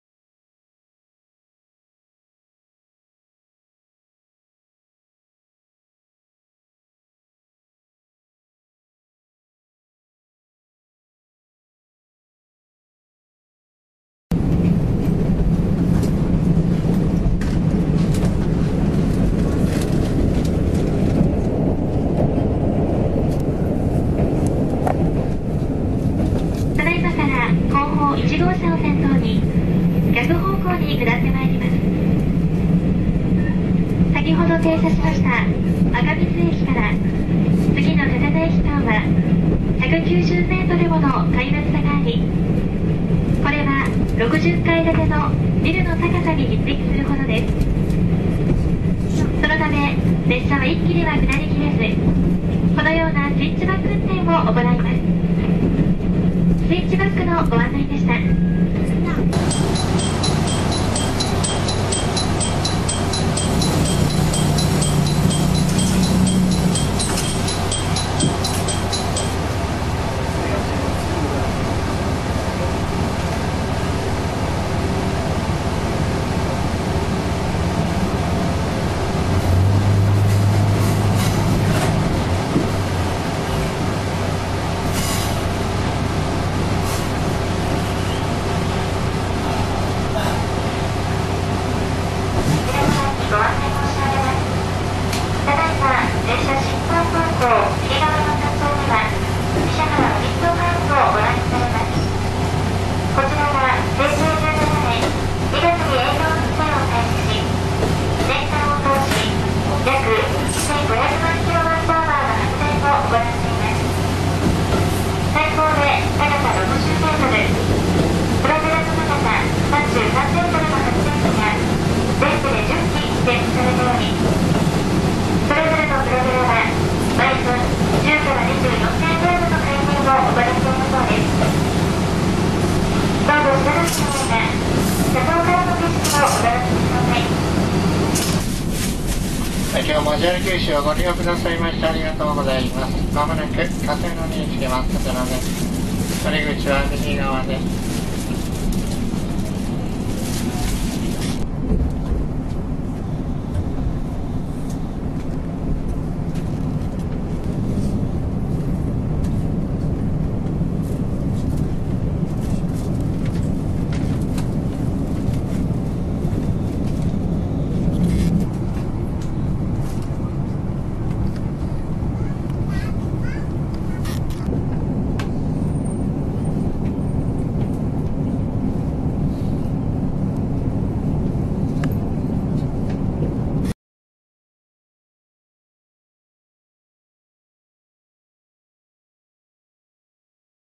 【熊本大地震】九州横断特急で阿蘇大橋西側崩れた崖の中を突っ走る
【熊本大地震】九州横断特急で阿蘇大橋西側崩れた崖の中を突っ走る４年前のビデオを再編集しました20160612；見ているだけで恐ろしいです！